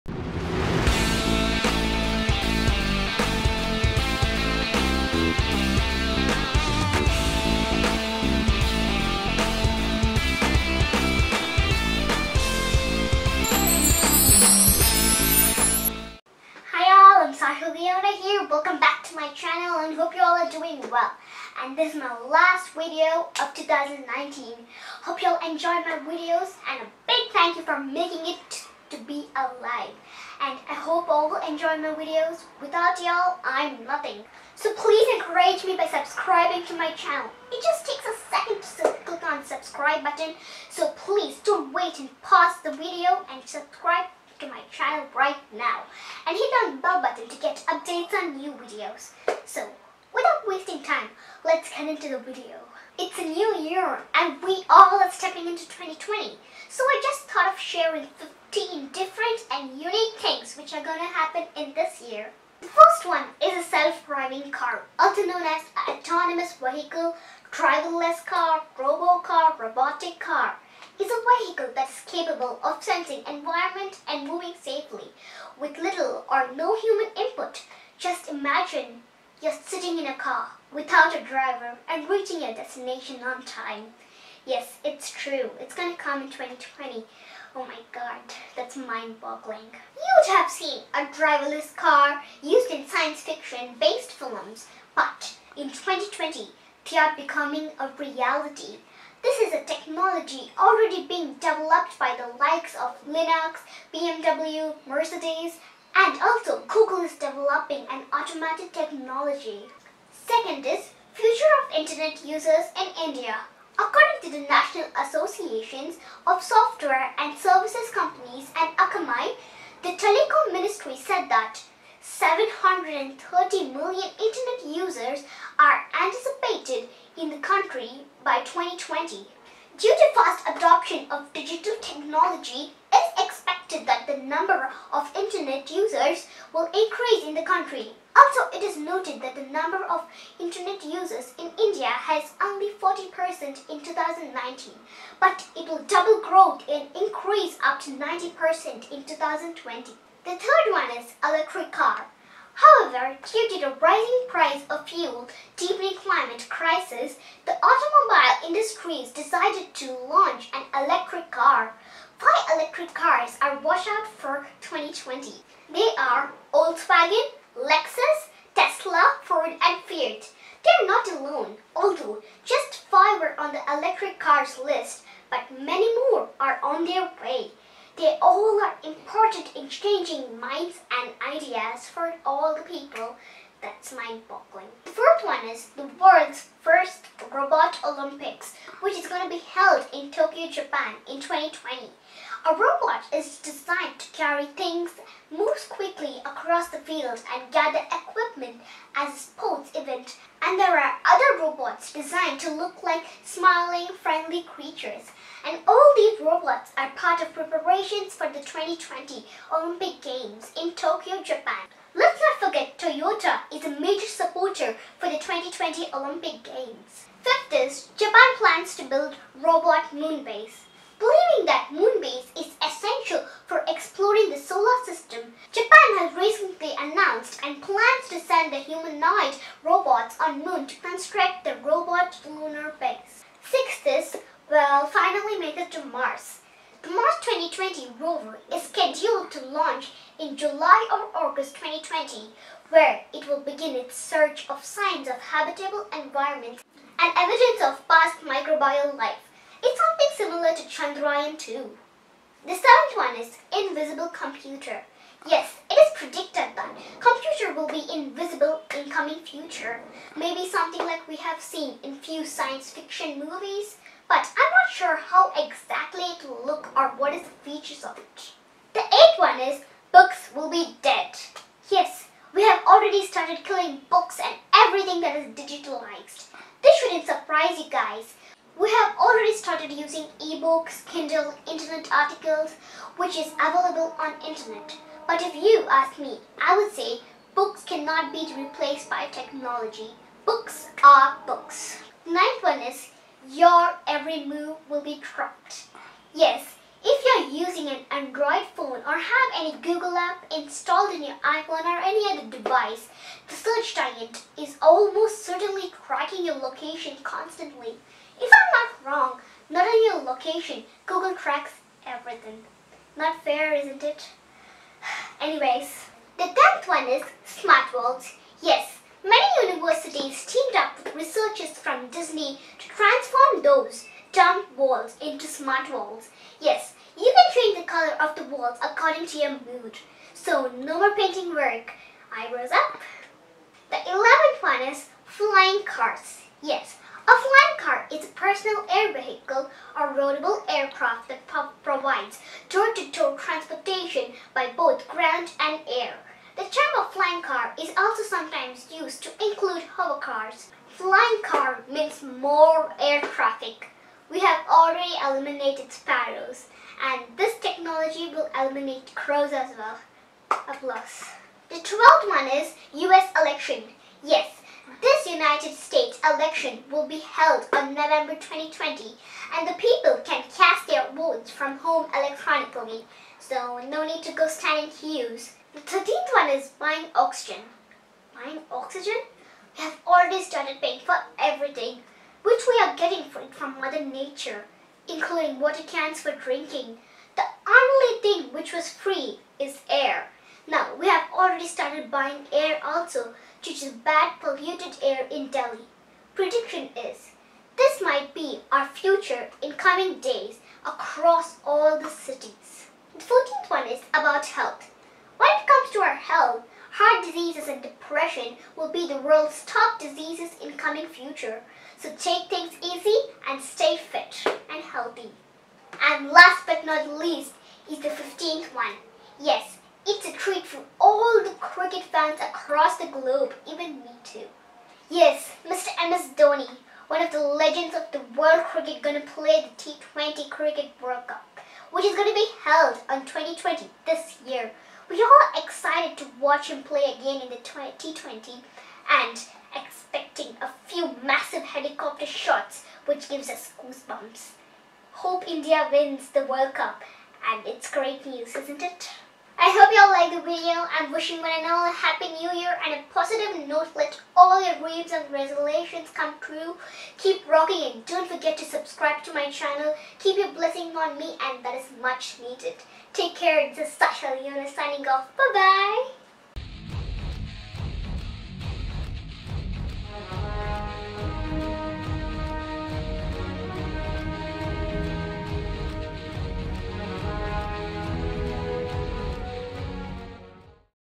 15 Things That Will Happen In 2020 | What is Future Technology | English | Sasha's Vlog
First one, a self-driving car,

Second is future of internet users in India.

Third is electric car.

Fourth is world’s first robot Olympics which is going to be held in Tokyo, Japan in 2020.

Fifth is Japan plans to build robot moon base.

Sixth is We’ll finally make it to mars.

Seventh one is Invisible computer,

Eighth one Books will be dead,

Ninth one is, your every move will be tracked,

The tenth one is, Smart Walls,

Eleventh one is Flying cars;

Twelfth one is US election,

Thirteenth one is buying oxygen,

Fourteenth one is about health, when it comes to health, heart disease and depression will be the world’s top diseases in coming future. So take things easy and stay fit and healthy.

And the last but not the least fifteenth one; it’s a treat for all the cricket fans across the globe even me too, Yes Mr. MS Dhoni one of the legends of world cricket gonna play the T20 cricket world cup which will be held on 2020 this year.